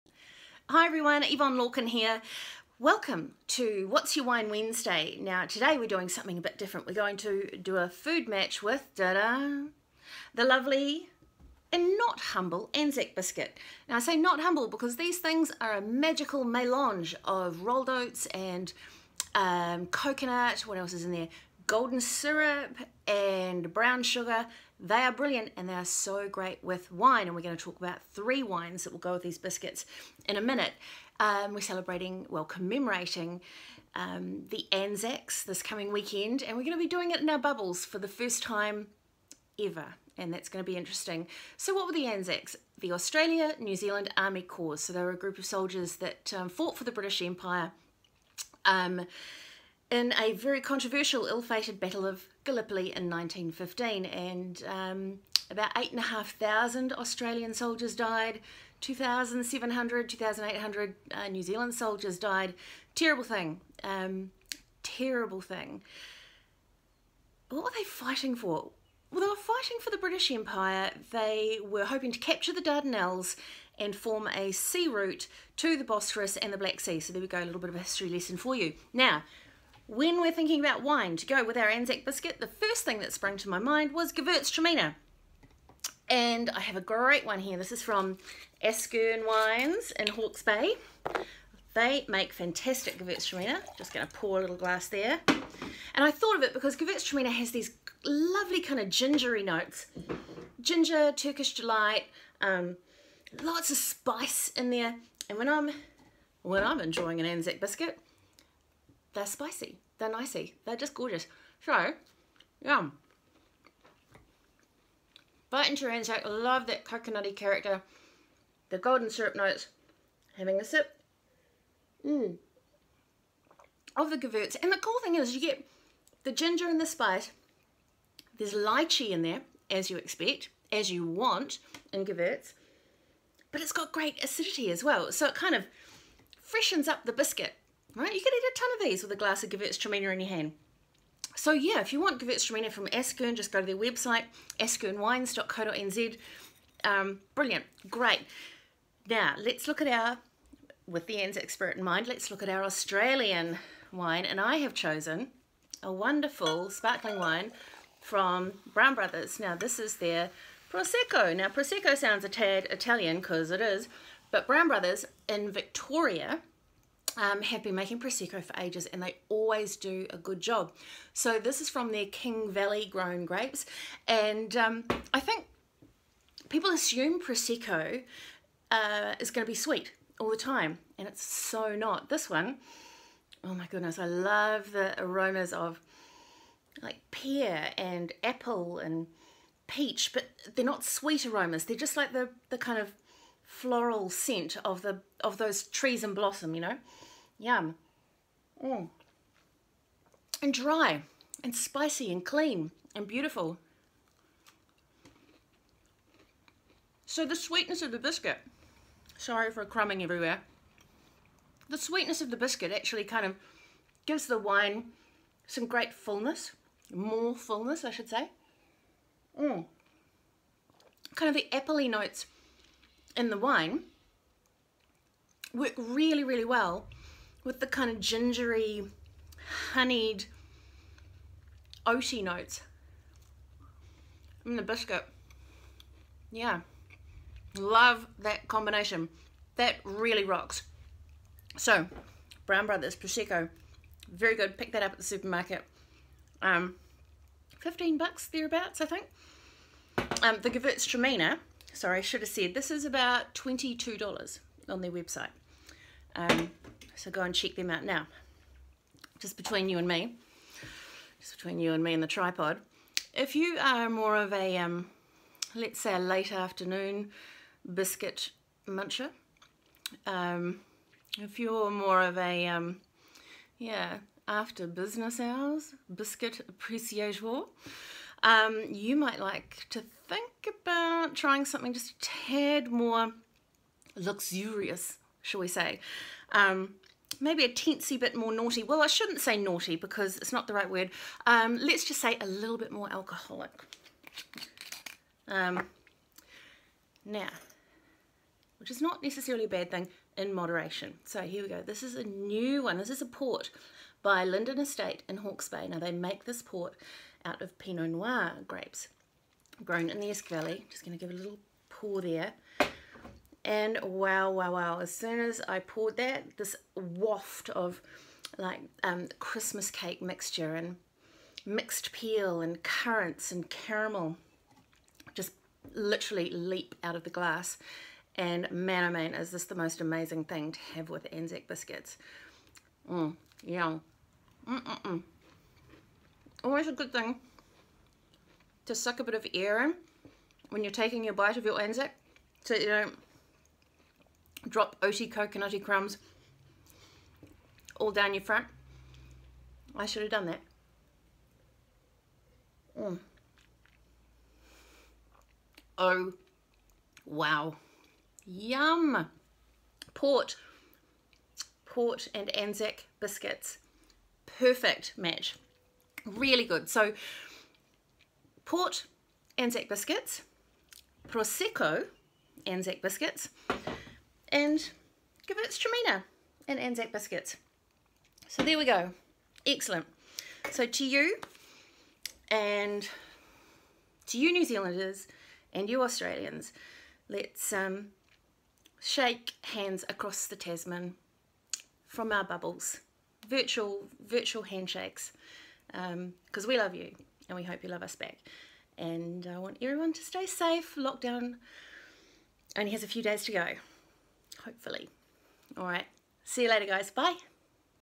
What's Your Wine Wednesday April 22 2020
Iiiiiiiiiit's What's Your Wine Wednesday chums! Today I've paired the awesomely oaty, chewy, crumb-tasticness of the ANZAC bikkie with wines from Askerne Estate Winery Brown Brothers Winery and Linden Estate Winery Make sure you've got the sound ON and sharing is caring cobbers!